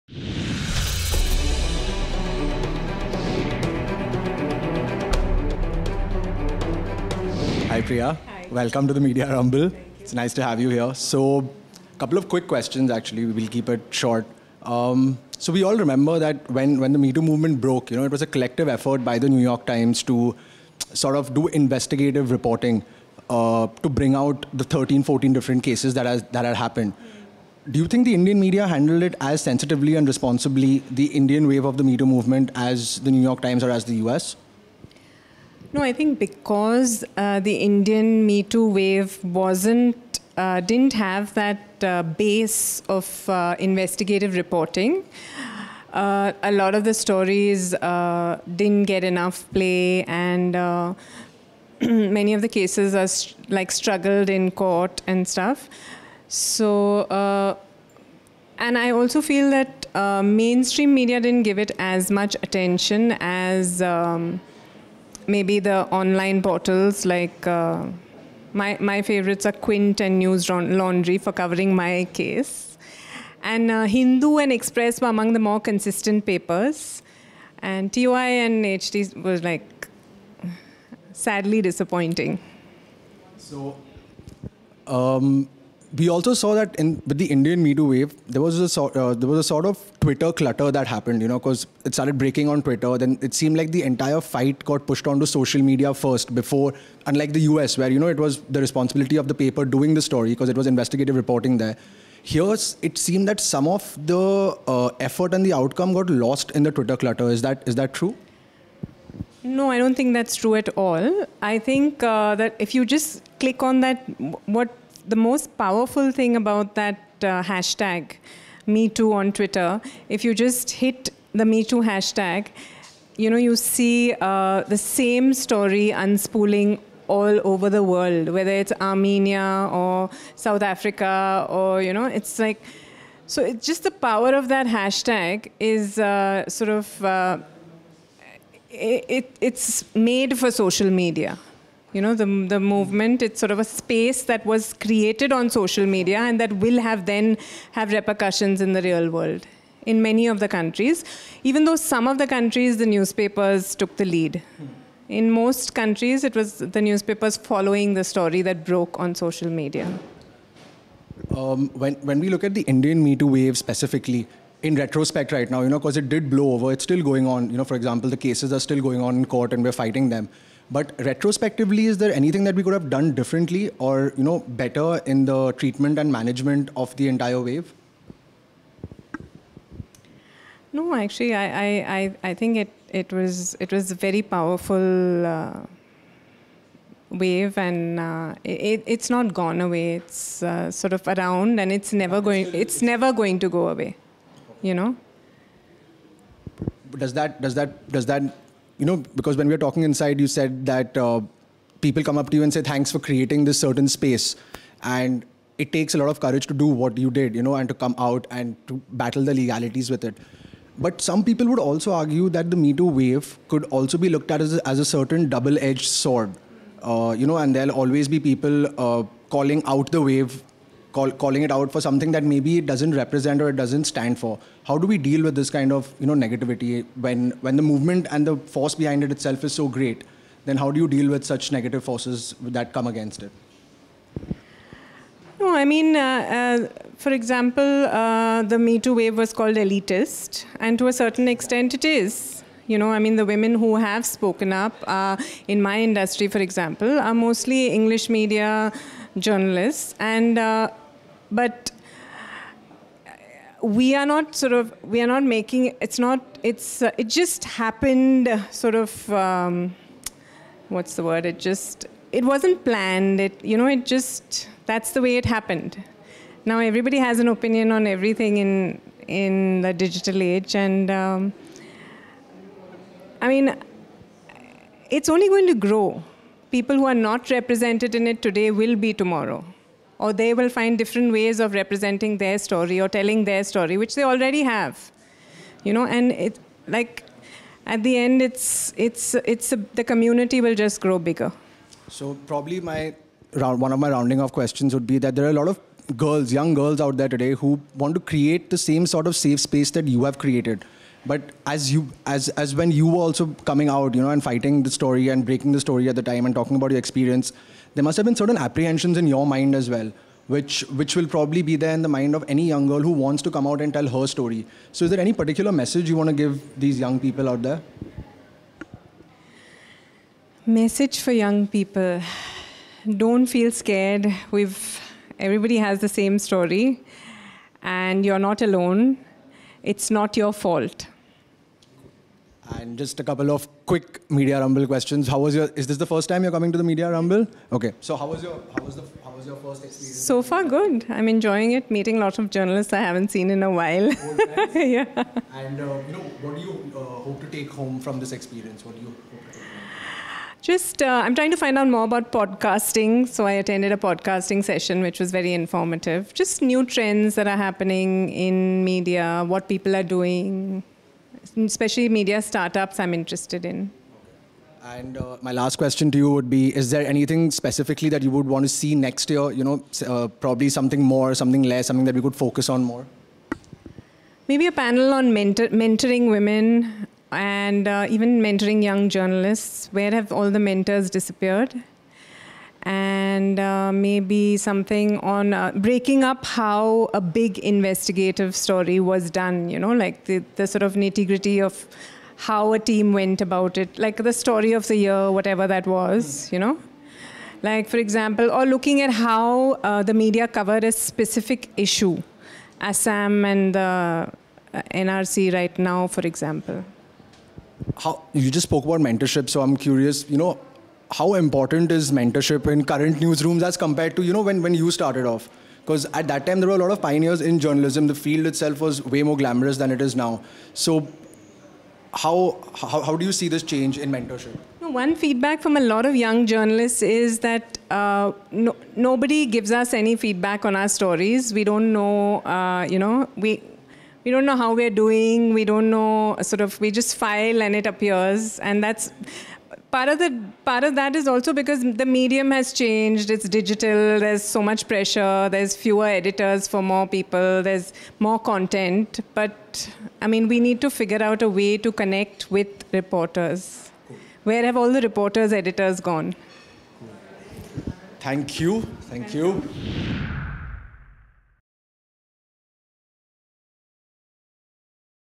MediaRumble interview: Priya Ramani on MeToo movement
On the sidelines of The Media Rumble, Gaurav Sarkar interviews journalist Priya Ramani. Ramani writes a weekly column for Mint and is a contributing editor at Vogue. When the Metoo movement broke out, Ramani accused former Union minister, M.J. Akbar of sexual harassment in a series of tweets.